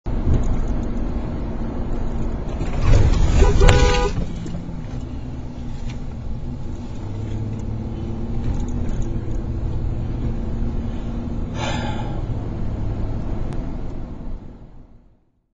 Vehicle Wreck Near Miss Dashcam Footage
Driving home from work, a car pulls out in front of me on River Ave in Pittsburgh.

I'm driving a 2004 Ford Ranger.  Dashcam is a modified Logitech webcam mounted behind my rear-view mirror.